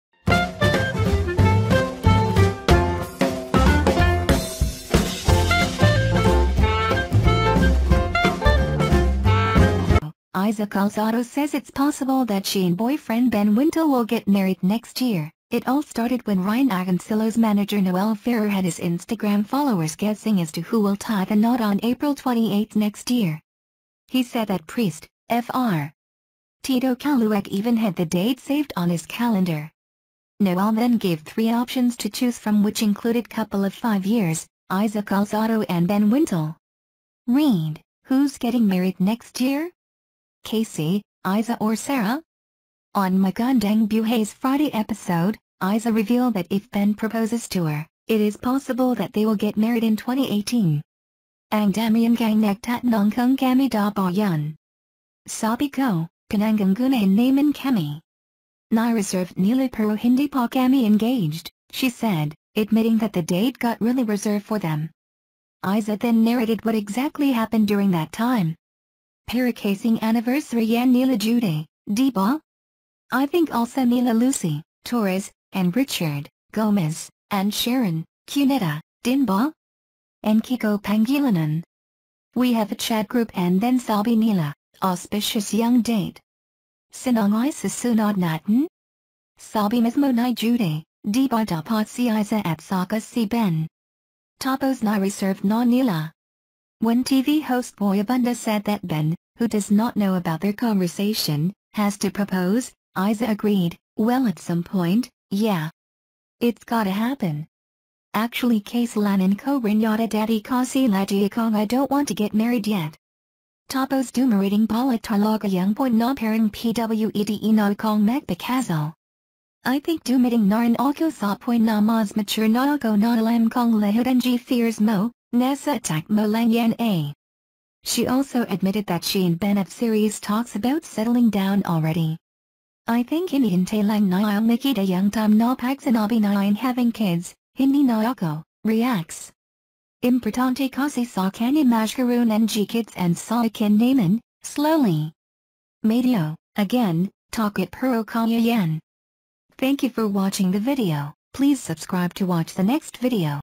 Iza Calzado says it’s possible that she and boyfriend Ben Wintle will get married next year
It all started when Ryan Agoncillo’s manager Noel Ferrer had his Instagram followers guessing as to who will tie the knot on April 28 next year. He said that priest, Fr. Tito Caluag even had the date saved on his calendar. Noel then gave three options to choose from which included couple of five years, Iza Calzado and Ben Wintle.

READ: Who’s getting married next year? KC, Iza or Sarah?

On Magandang Buhay’s Friday episode, Iza revealed that if Ben proposes to her, it is possible that they will get married in 2018.

“Ang dami ngang nagtatanong kung kami daw ba ‘yun. Sabi ko, pinangungunahan naman kami. Nireserve nila pero hindi pa kami engaged,” she said, admitting that the date got really reserved for them.